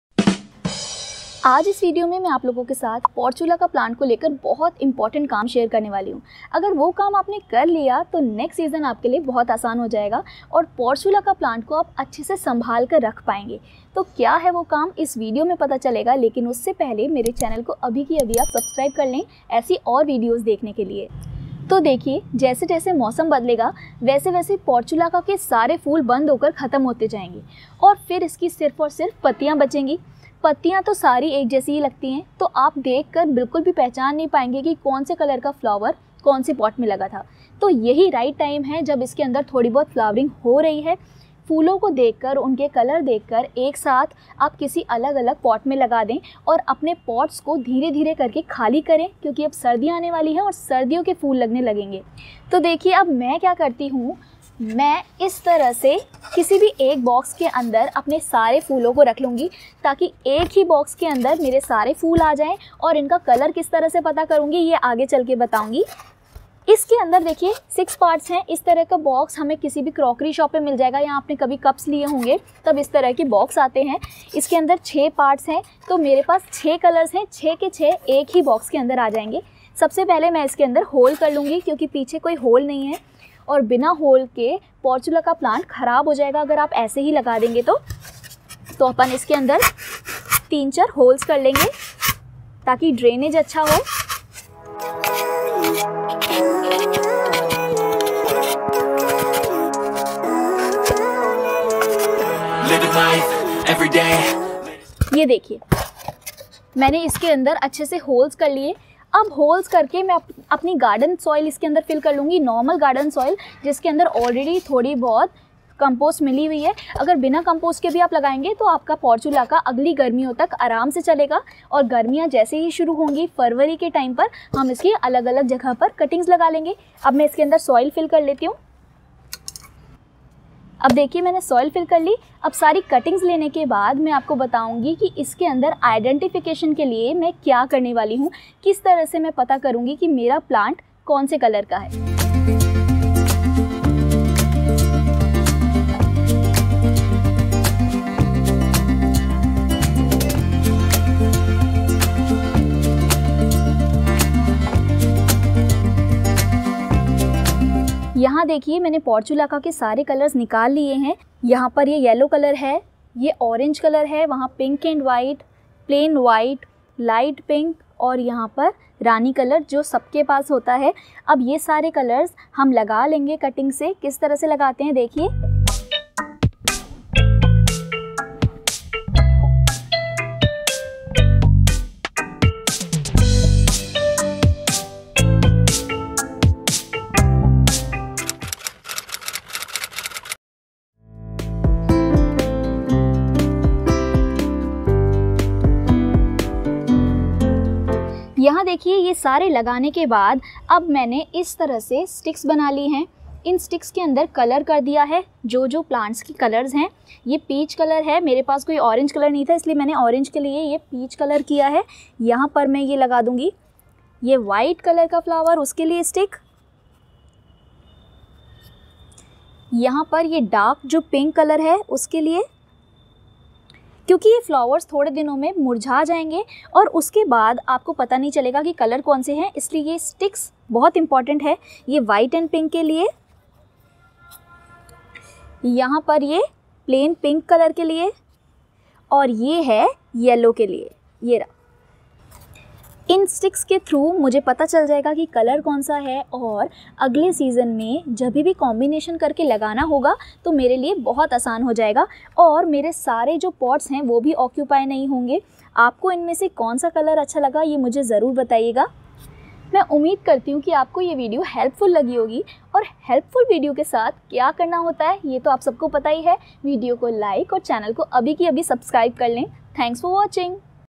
Save This Portulaca Plant for Next Season - A Quick and Easy Guide
This video is a quick and easy guide for saving the Portulaca plant for next season. In winter, the Portulaca plant stops blooming its flowers, so we will be unable to identify the colours of Portulaca flowers in next season. To get rid of this problem, I am sharing tips and tricks to save Portulaca for next season.